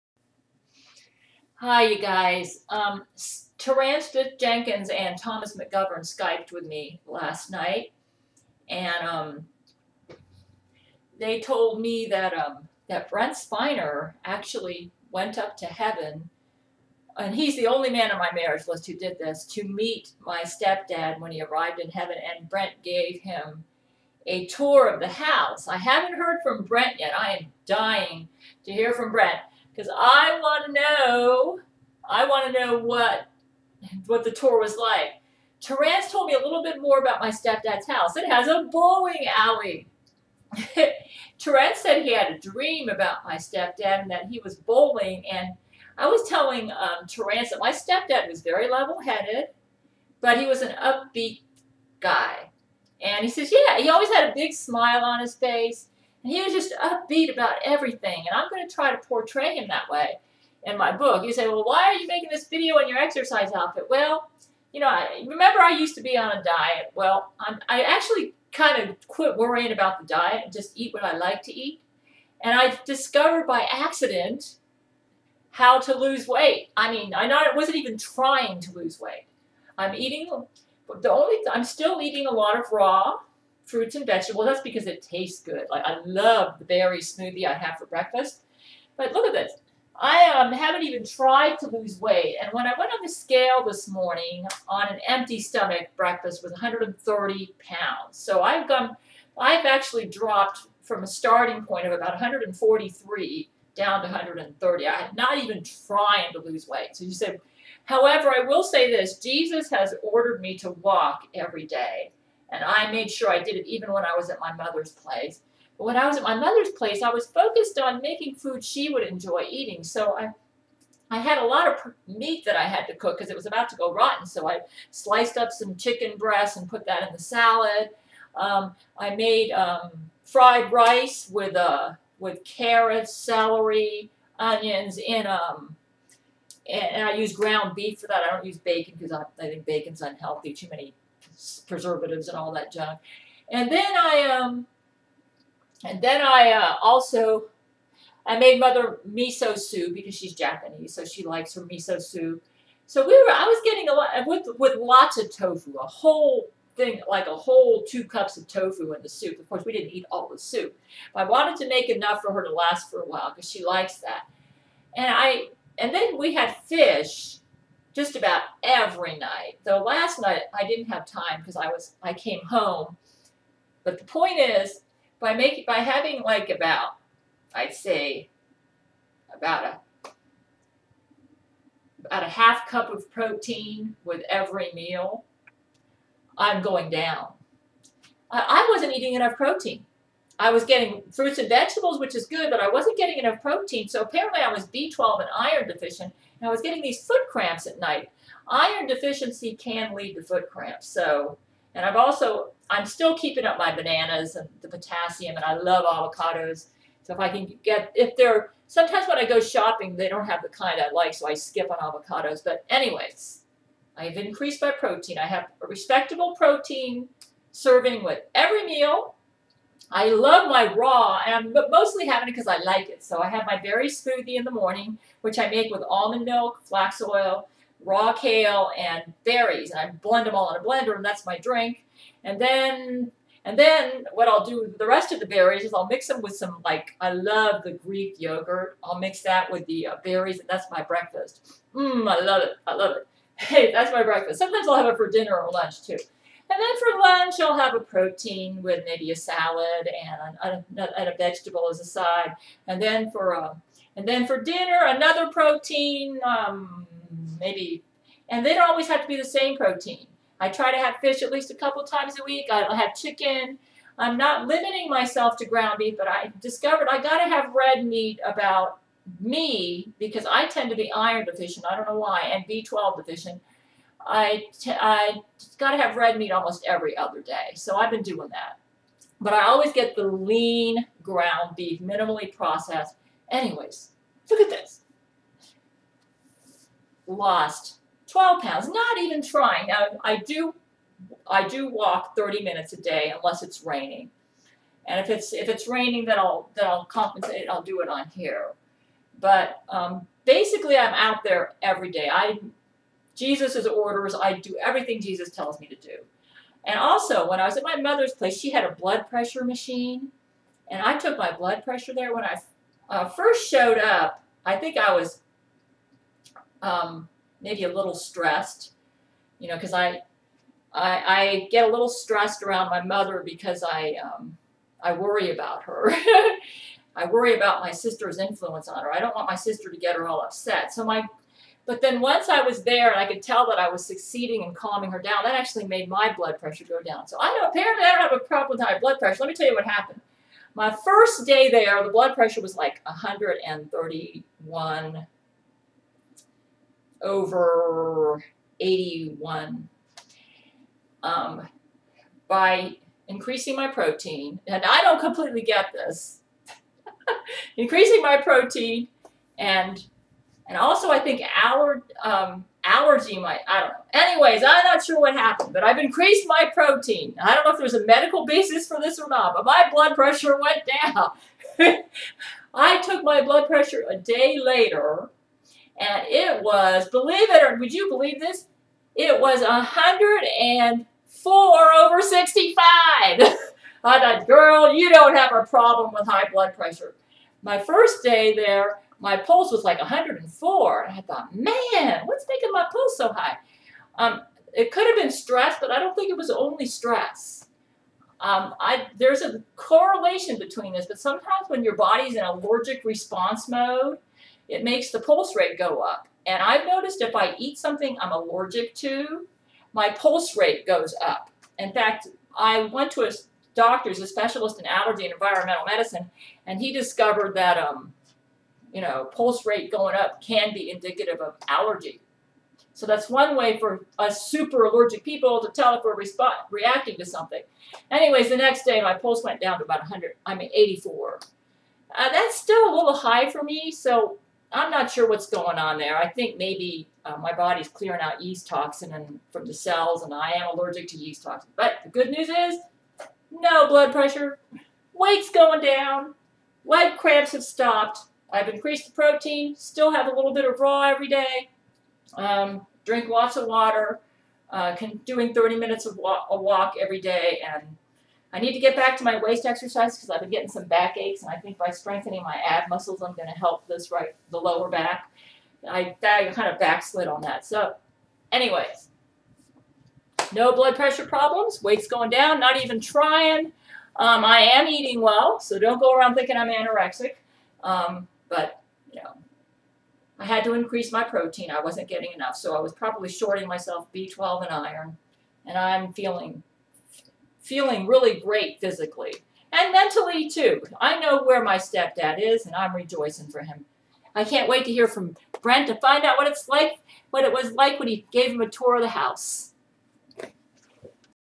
Step Dad Tours New Home in Heaven
Gail describes her day and bits and pieces of the Skype conversation she had with Tom McGovern and Terrance Jenkins on the evening of March 12th, the day her step dad entered heaven. She also explains how she got rid of her foot cramps, and how she went from 143 pounds to about 131 pounds, without even trying too hard.